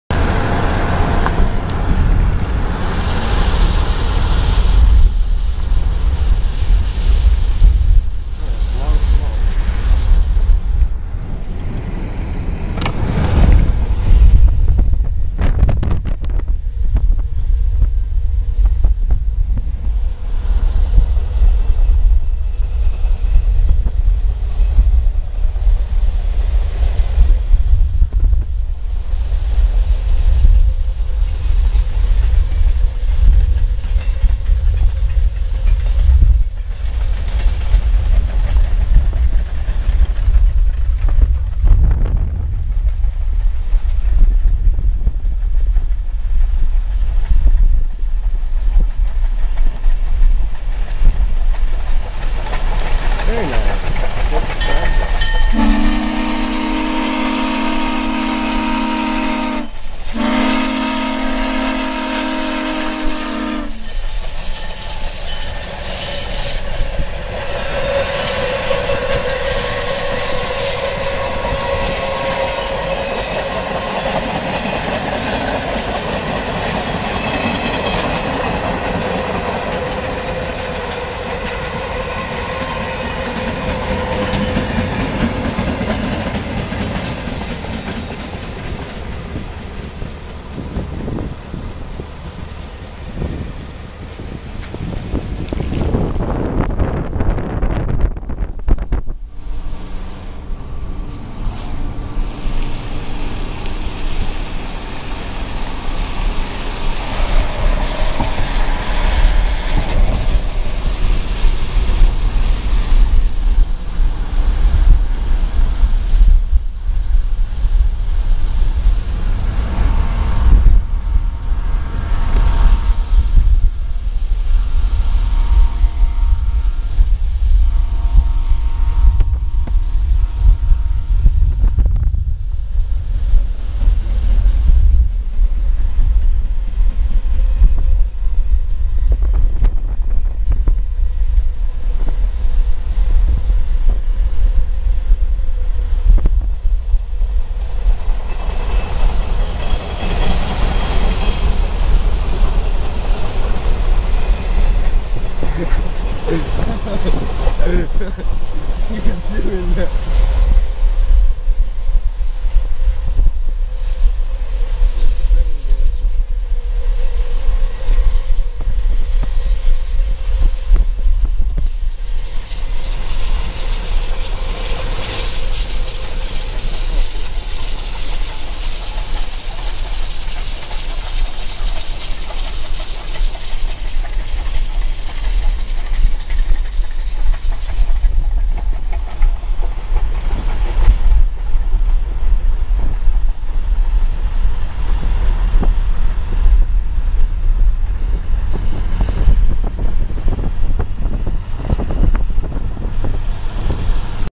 IAIS #7081 steam chaste part 1
IOWA INTERSTATE #7081 chaste between Wiota & Anita IA part 1